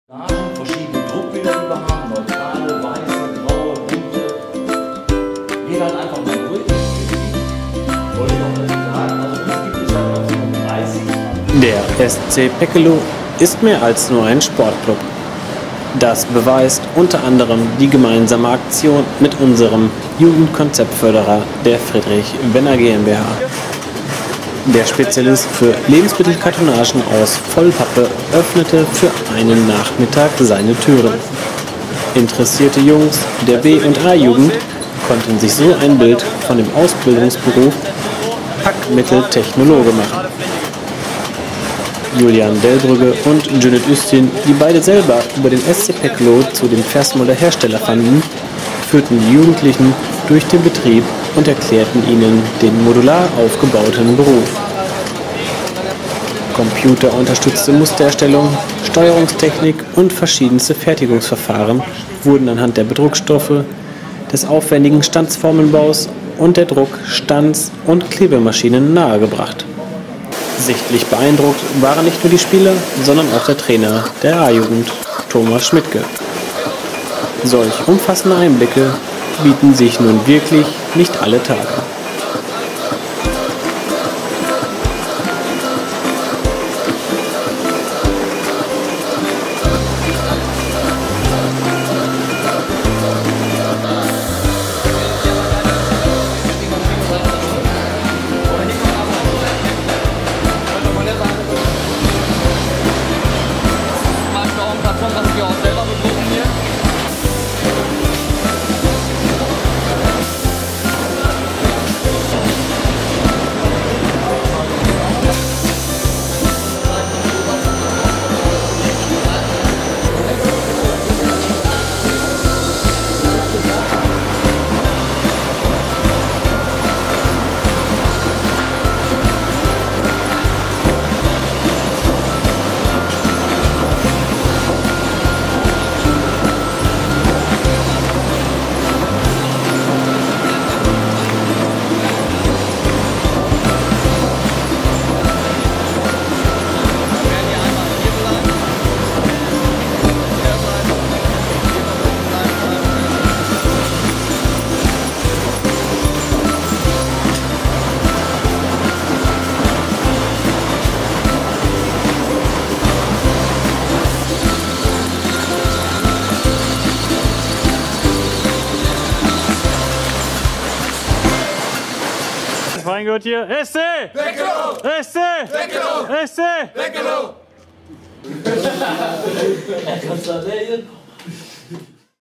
Der SC Peckeloh zu Gast bei der Friedrich Wenner GmbH
„Der SC Peckeloh ist mehr als nur ein Sport-Club!"
Das beweist unter anderem die gemeinsame Aktion mit einem unserer Jugendkonzeptförderer, der Friedrich Wenner GmbH.
Der Spezialist für Lebensmittelkartonagen aus Vollpappe öffnete für einen Nachmittag seine Türen.
Interessierte Jungs der B- und A-Jugend konnten sich so ein Bild von dem Ausbildungsberuf „Packmitteltechnologe" machen.